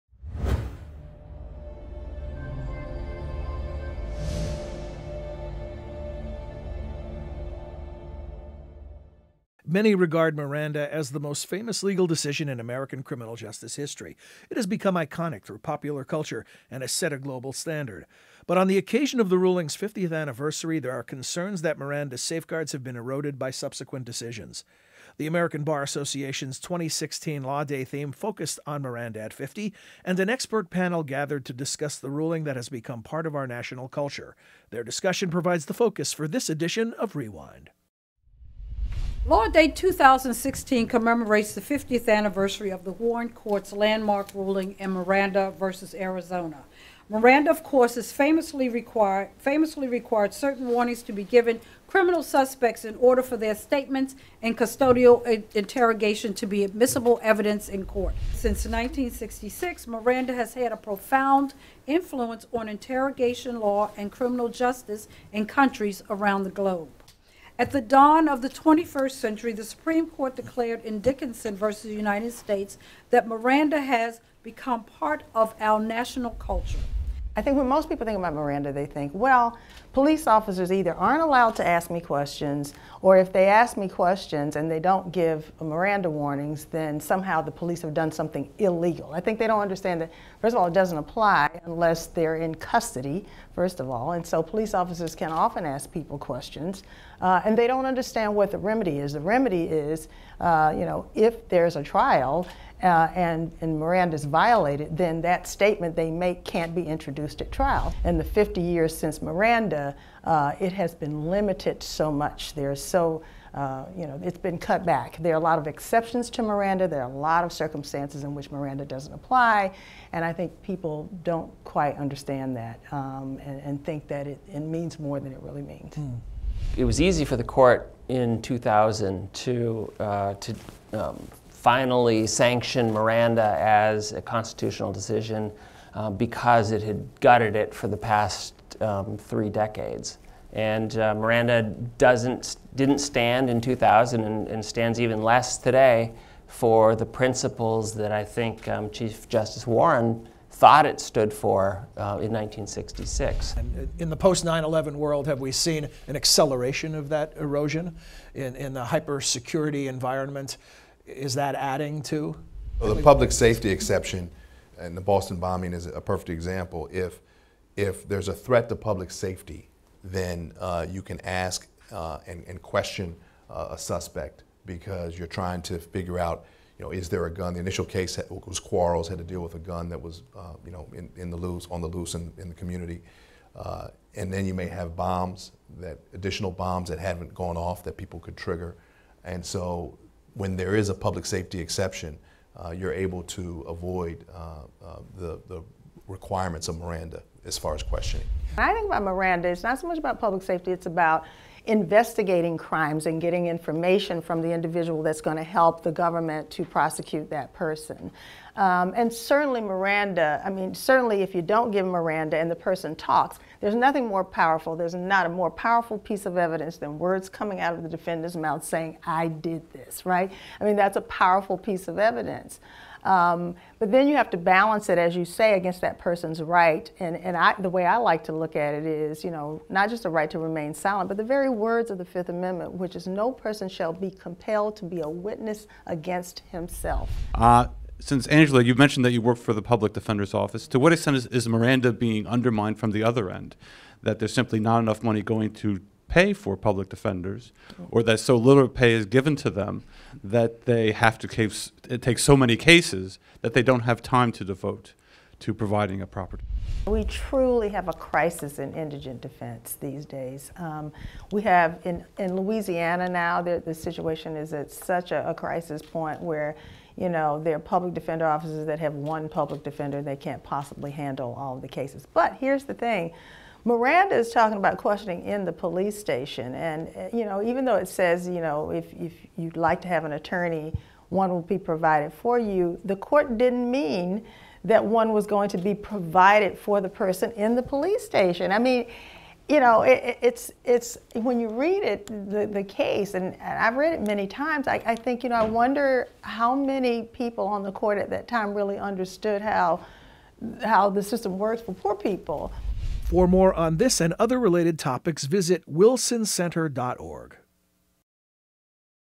Law Day 2016 – Miranda: More Than Words
The Miranda ruling is likely the most iconic legal decision in US history. It’s been 50 years since Miranda v. Arizona, and to commemorate the anniversary, the American Bar Association’s Law Day theme focused on the landmark case. Many believe that its protections have been weakened by subsequent rulings and that understanding of its meaning is superficial, driven mainly by popular culture portrayals. An expert panel discussed the status of current Miranda during the annual ABA Leon Jaworski Public Program conducted at the Wilson Center. That’s the focus of this edition of REWIND.

Speakers (listed in order of appearance)
Paulette Brown
President, American Bar Association

Angela J. Davis
Professor of Law, American University Washington College of Law

Russell Dean Covey
Professor of Law, Georgia State University

John Milewski
Executive Producer & Host, Wilson Center ON DEMAND

Ronald C. Machen
Partner, WilmerHale

William Pomeranz
Deputy Director, Kennan Institute